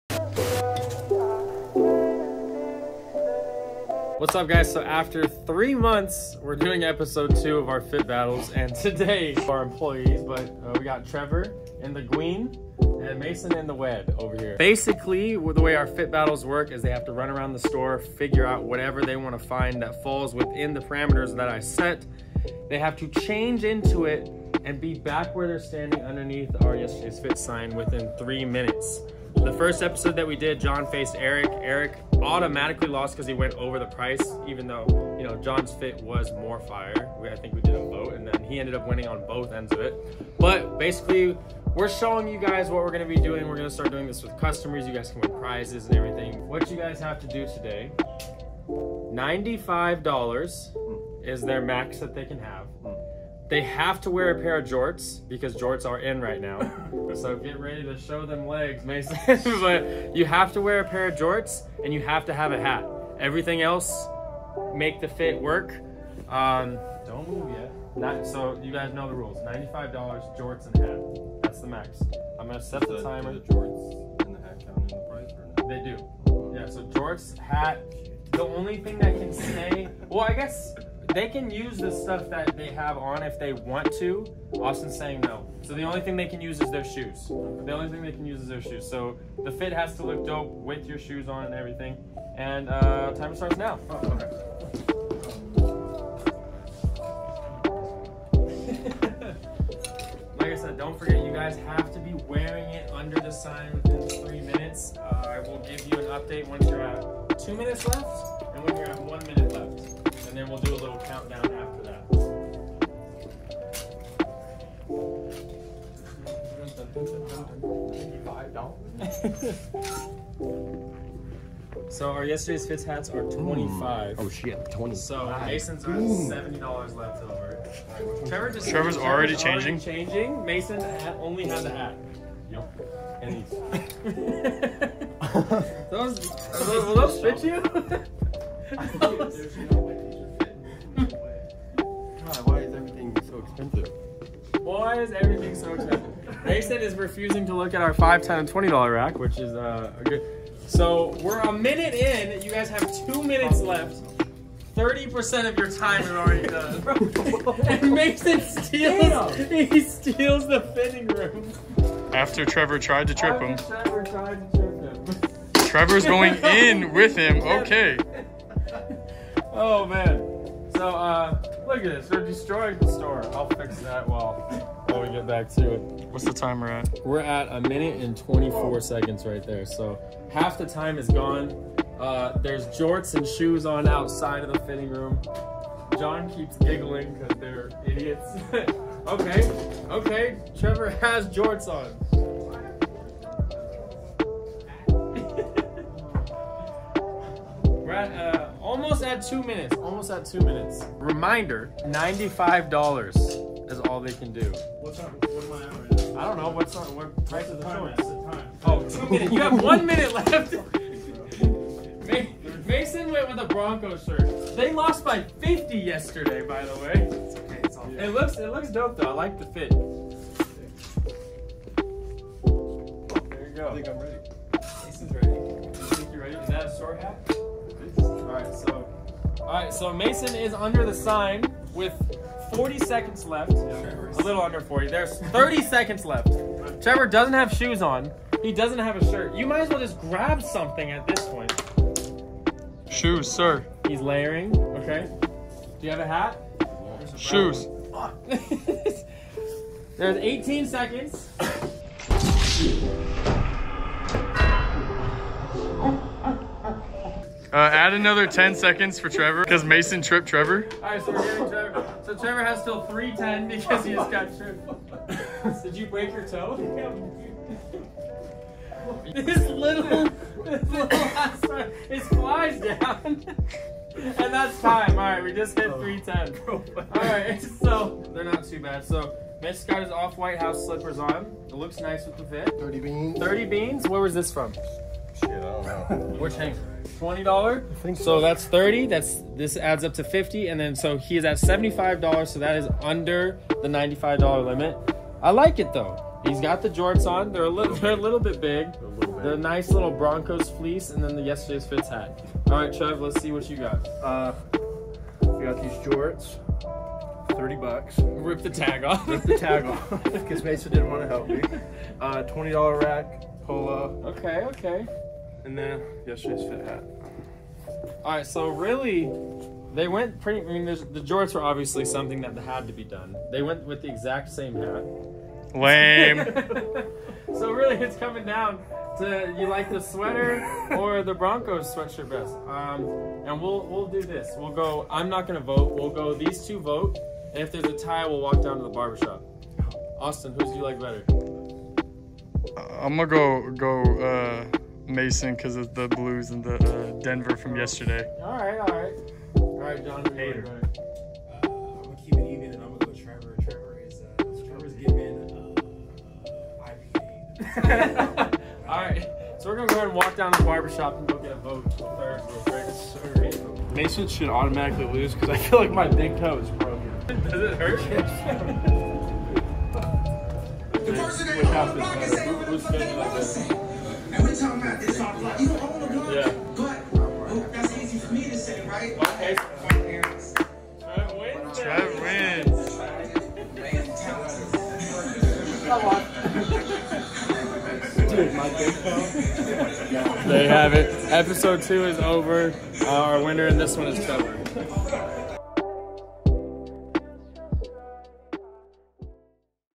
Gotta Go Fast | Yesterday's Fits Fit Battles EP 2
Welcome to the official Yesterday's Fits Youtube Channel! ✨

Today, Mason and Trevor compete in a fit battle in the shop to create an entire fit and put it on! The boys rush to see what items they could use and show off their fits!

If you are reading this far in the description, comment whose fit was better, even though ! :D